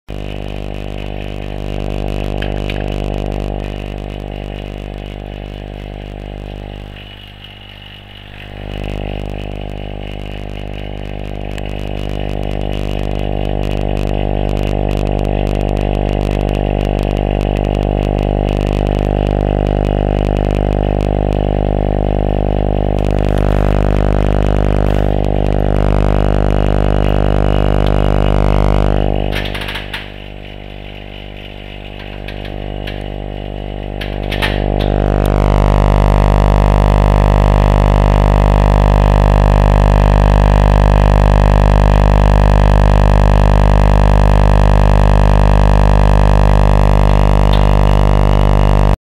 My Little Star...
Vakuum pump and vakuum chamber test.
DC 10kV maybe 10mA. The accelerator diameter @15X25mm Plasma ball 10mm

I think I made a pretty light bulb. :)